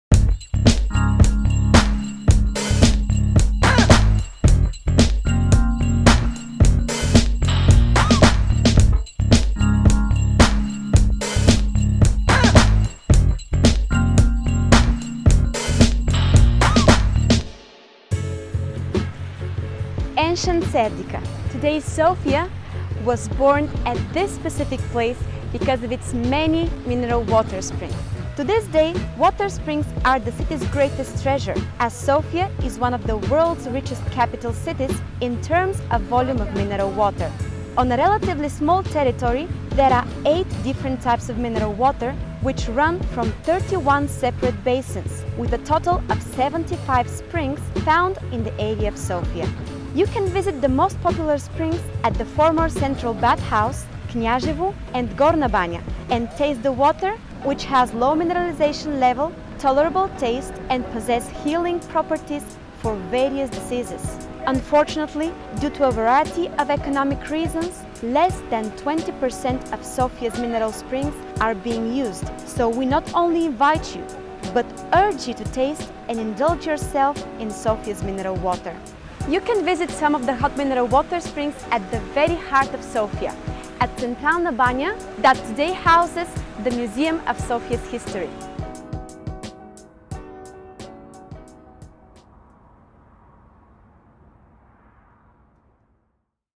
soSofia - soEurope — Sofia's Mineral Water
soSofia - soEurope is an initiative to promote the short stories of seven of Sofia's most emblematic symbols and their emergence and imposition in the context of the shared European history.